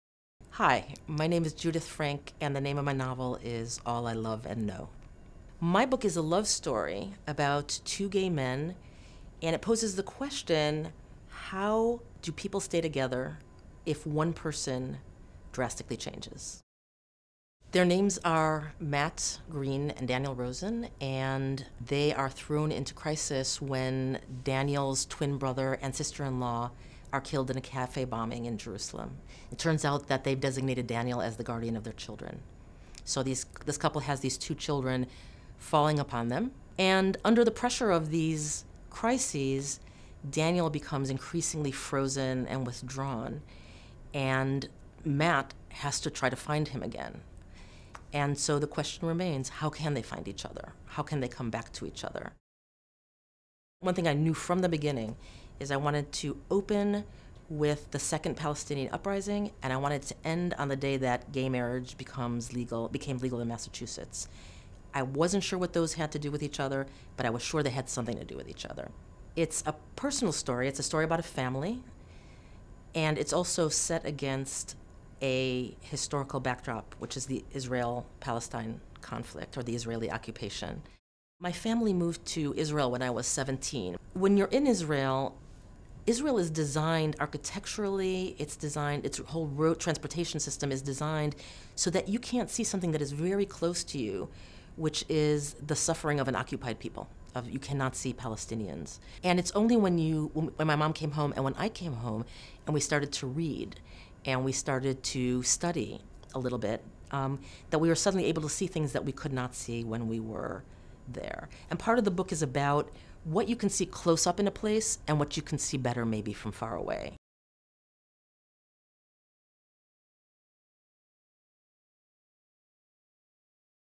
Al I Love and Know, a novel by Judith Frank
With the storytelling power of Wally Lamb and the emotional fidelity of Lorrie Moore, this is the searing drama of an American family on the brink of dissolution, one that explores adoption, gay marriage, and true love lost and found.
For years, Matthew Greene and Daniel Rosen have enjoyed a contented domestic life in Northampton, Massachusetts. Opposites in many ways, they have grown together and made their relationship work. But when they learn that Daniel's twin brother and sister-in-law have been killed in a Jerusalem bombing, their lives are suddenly, utterly transformed.
The deceased couple have left behind two young children, and their shocked and grieving families must decide who will raise six-year-old Gal and baby Noam. When it becomes clear that Daniel's brother and sister-in-law had wanted Matt and Daniel to be the children's guardians, the two men find themselves confronted by challenges that strike at the heart of their relationship. What is Matt's place in an extended family that does not completely accept him or the commitment he and Daniel have made? How do Daniel's complex feelings about Israel and this act of terror affect his ability to recover from his brother's death? And what kind of parents can these two men really be to children who have lost so much?
The impact that this instant new family has on Matt, Daniel, and their relationship is subtle and heartbreaking, yet not without glimmers of hope. They must learn to reinvent and redefine their bond in profound, sometimes painful ways. How does a family become strong enough to stay together and endure when its very basis has drastically changed? And are there limits to honesty or commitment—or love?